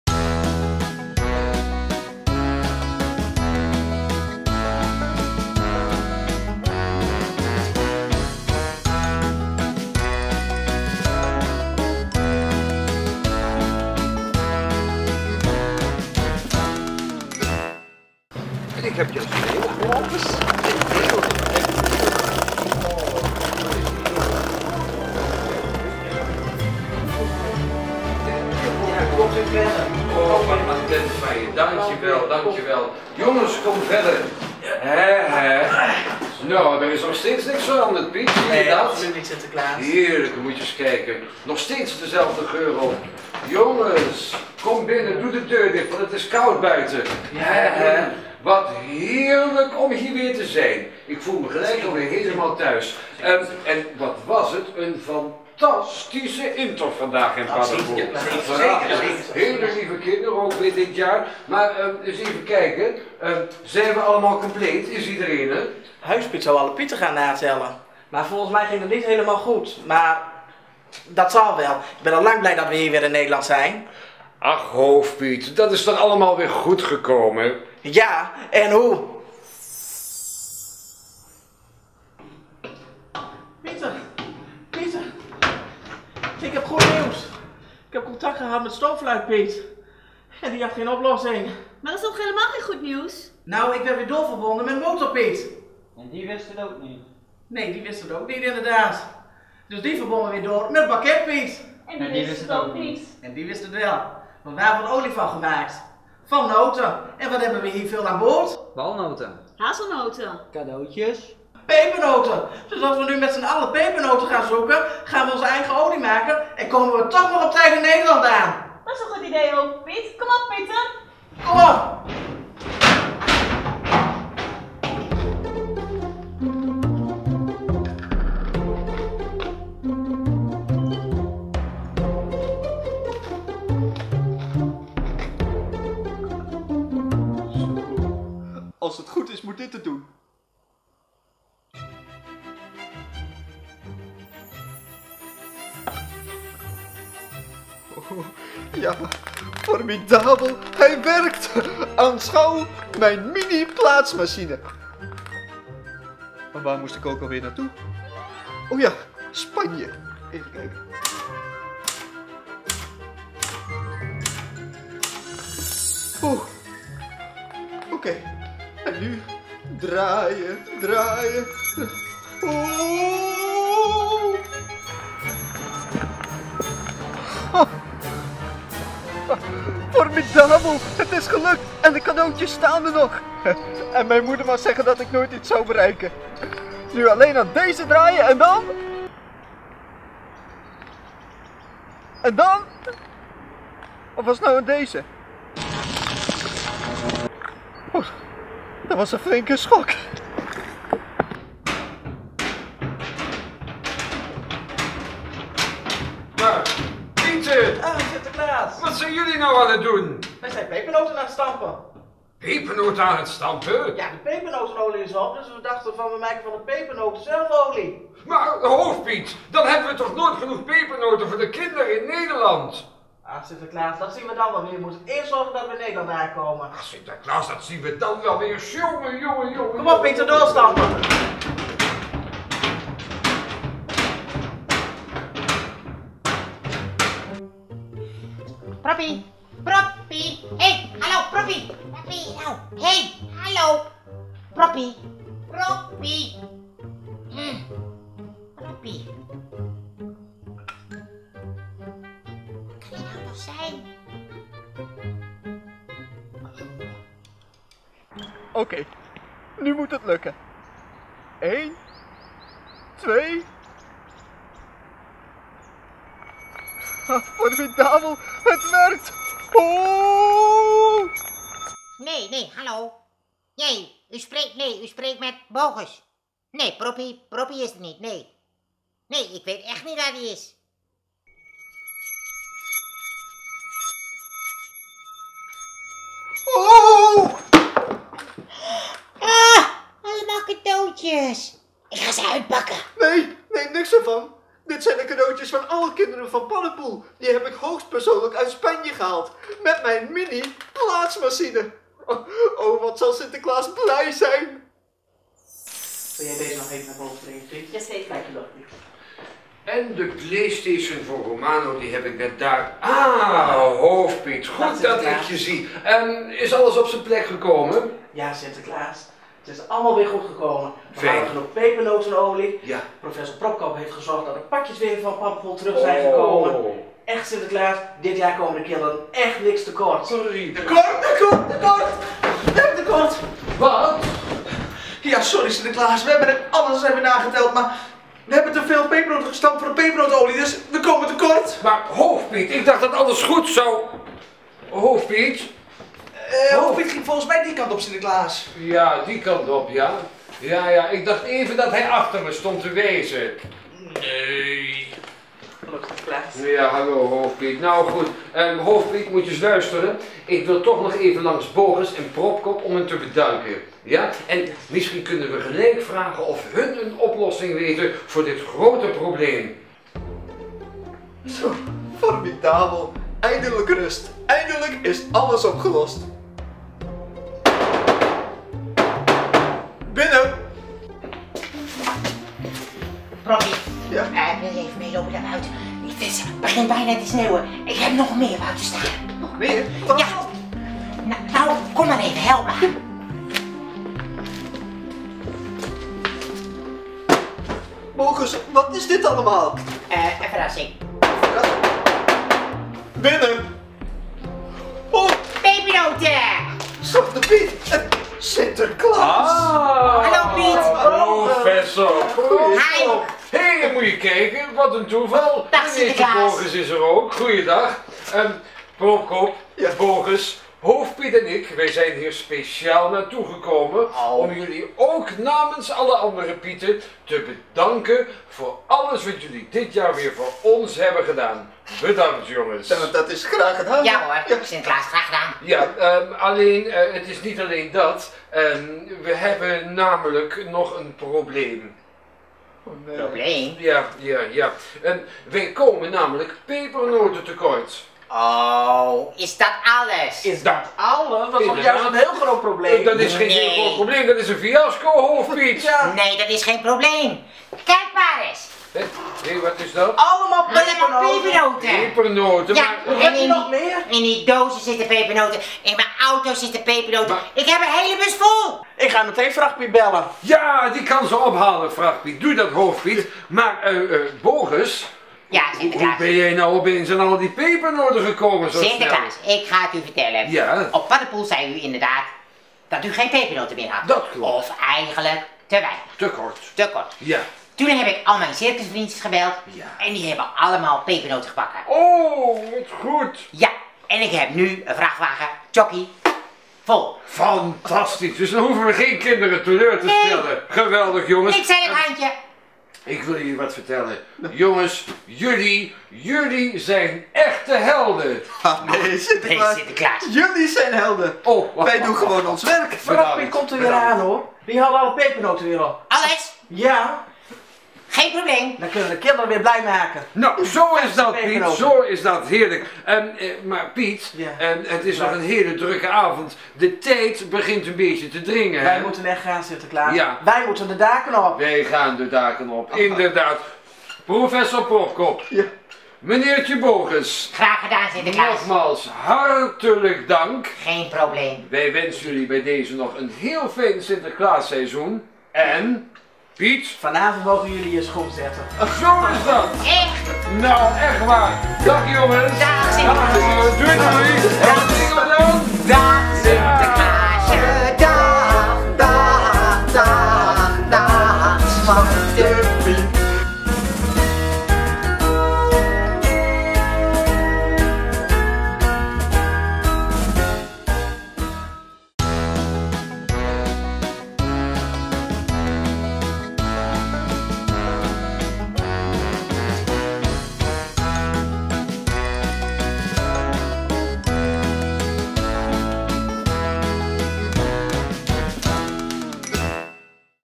Sinterklaas op Paddepoel - Aflevering 3. Sinterklaasserie 2018.
In: ''Sinterklaas op Paddepoel'' bereiden Sinterklaas en de pieten zich voor op de intocht op winkelcentrum Paddepoel. Deze vind 21 november 2018 plaats en er mag uiteraard helemaal niks mis gaan. Maar ja, dat soort dingen heb je natuurlijk nooit zelf in de hand.